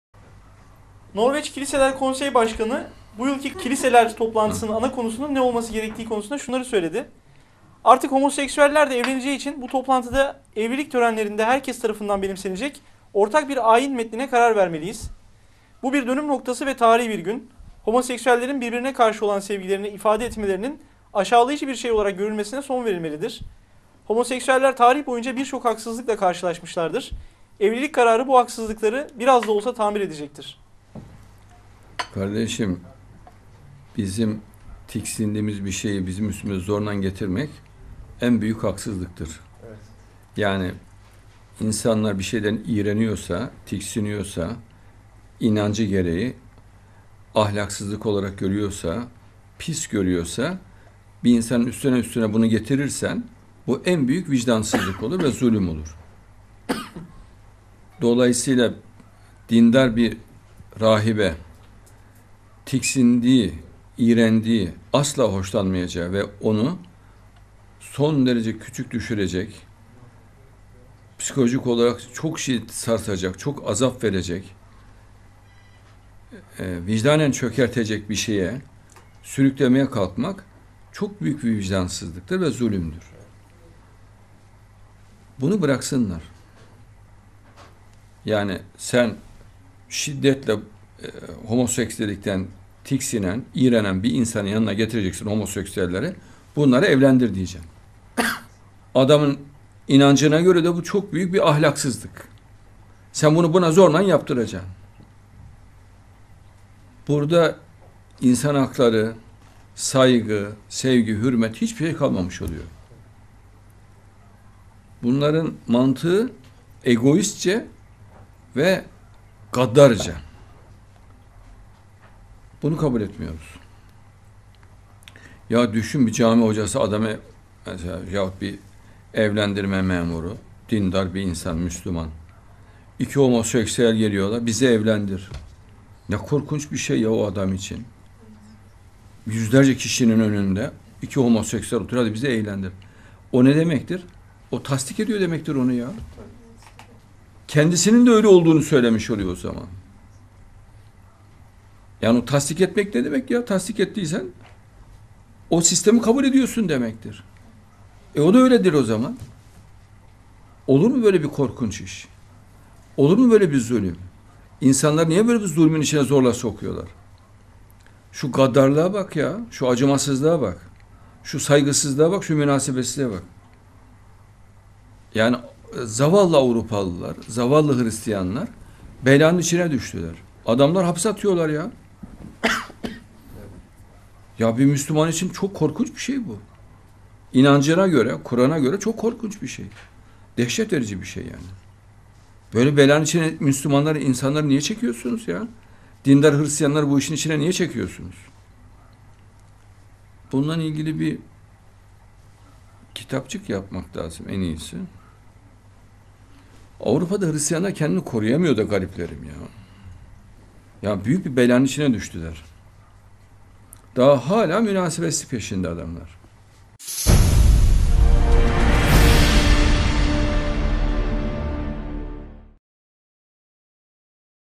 Norveç’te rahiplere “zorla homoseksüelleri evlendirin” denilmesi olmaz
Norveç’te rahiplere “zorla homoseksüelleri evlendirin” denilmesi olmaz.İnsanların inancına ters olan bir şeyi dayatmak zulümdür.

Adnan Oktar'ın Sohbetlerinden Seçme Bölümler

Sayın Adnan Oktar Ne Demişti Ne Oldu?